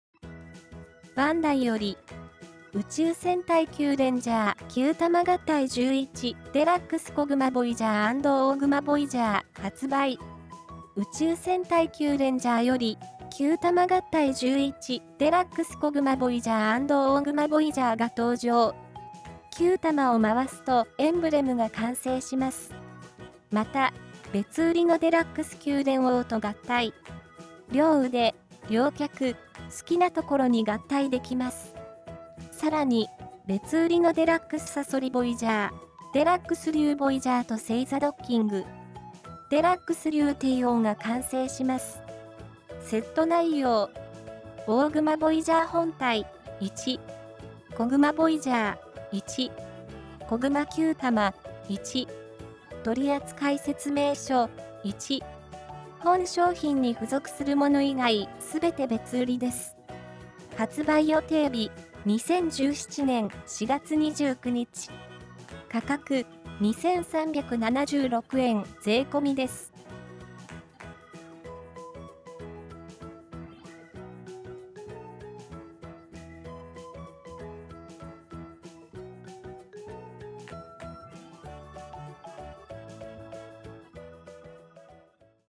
宇宙戦隊キュウレンジャー キュータマ合体11 DXコグマボイジャー＆オオグマボイジャー 発売!!
『宇宙戦隊キュウレンジャー』より、
「キュータマ合体11 DXコグマボイジャー＆オオグマボイジャー」が登場！

キュータマを回すとエンブレムが完成します。
また、別売りのDXキュウレンオーと合体！！両腕、両脚、好きなところに合体できます。
さらに、別売りのＤＸサソリボイジャー、ＤＸリュウボイジャーとセイザドッキング！
ＤＸリュウテイオーが完成します。

［セット内容］
・オオグマボイジャー本体…１
・コグマボイジャー…１
・コグマキュータマ…１
・取扱説明書…１

（本商品に付属するもの以外すべて別売りです。）

■宇宙戦隊キュウレンジャー キュータマ合体11 DXコグマボイジャー&オオグマボイジャー（リンク先：Amazon）